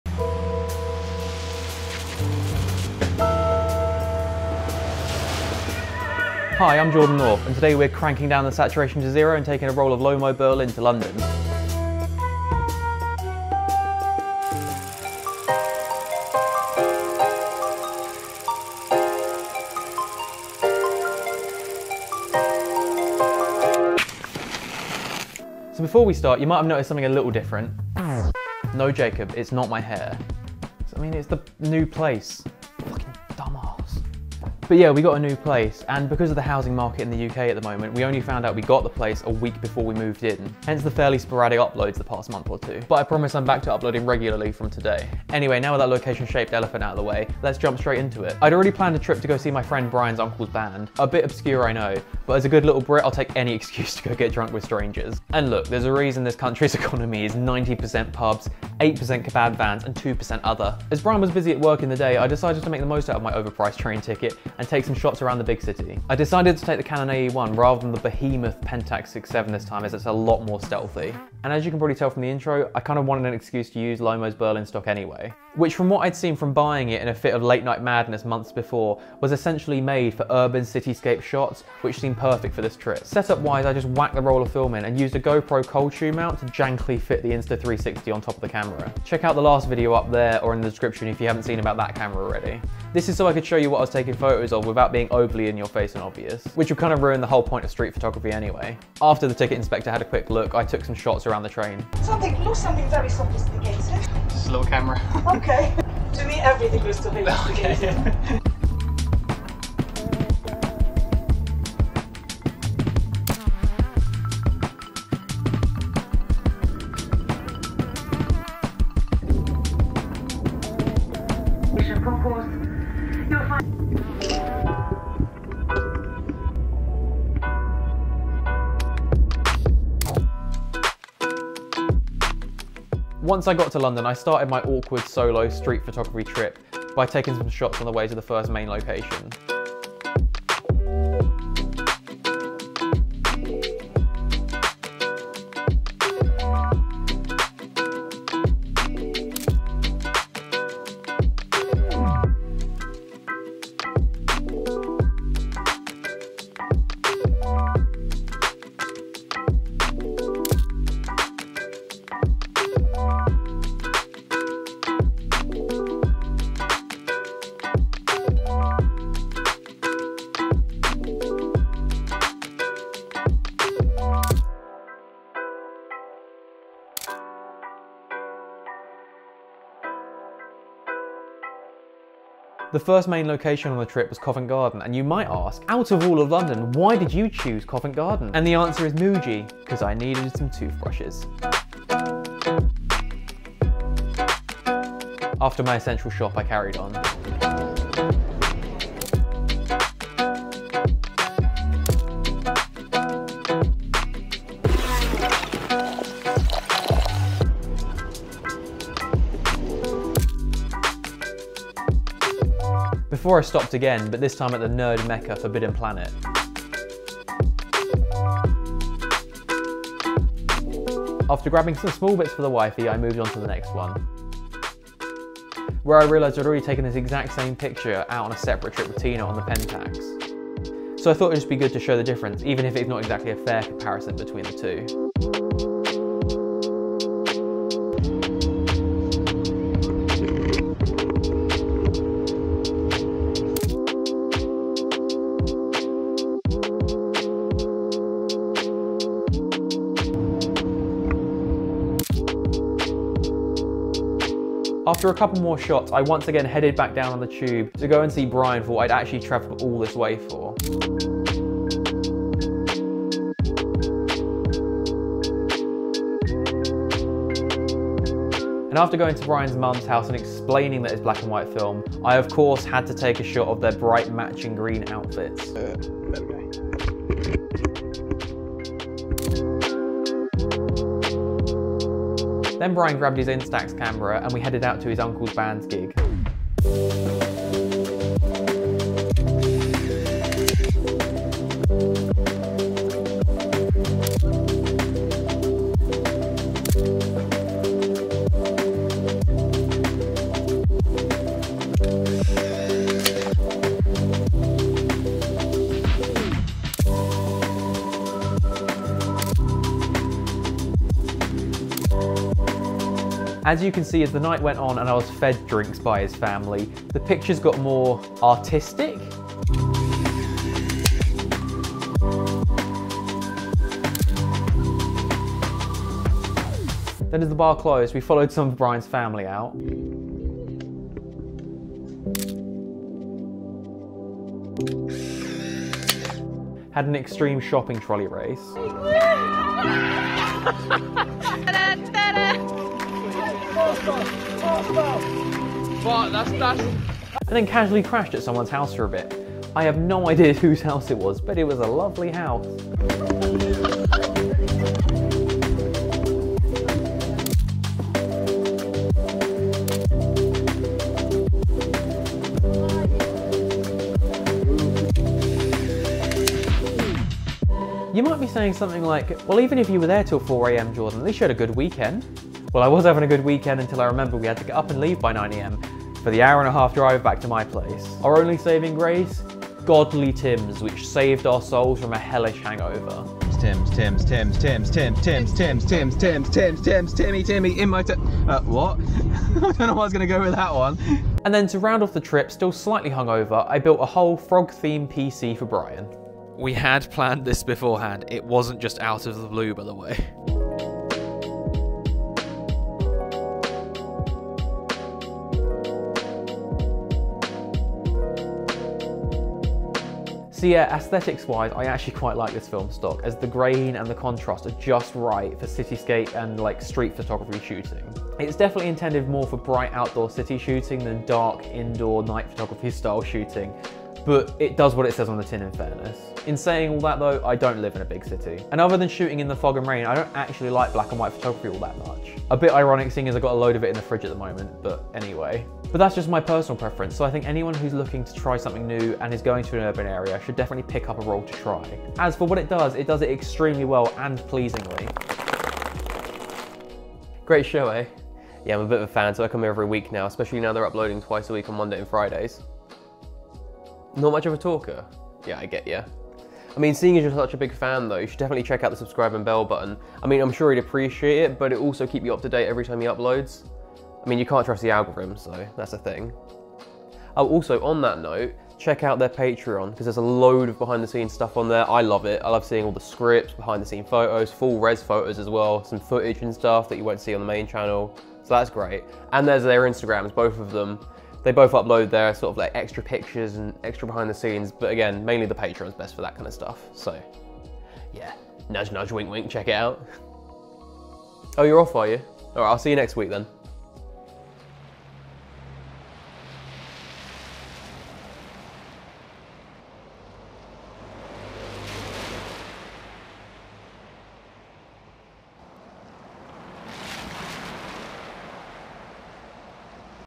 Lomo Berlin In London: Classic Street Photography With The Canon AE-1
Lomo Berlin In London, taking one of Lomo's famous black and white film stocks to the streets of London to put it through it's paces.

Vero: @21x9
Tinas Instagram: @eggyae1

Cameras used in and to make this video:
- Insta 360 one X2
- Canon AE-1
- Fujifilm Instax Mini 11

Chapters:
0:00 Titles
0:25 Intro
1:12 Film & Setup
1:40 The Photography Begins
2:12 Arriving In London
3:02 1st Location
3:45 Film Comparison
4:14 2nd Location Bryan
4:42 The Party Begins
5:26 After Party
6:08 Hungover Home Time
6:30 Frog PC
6:50 Thoughts On The Film
7:25 Outro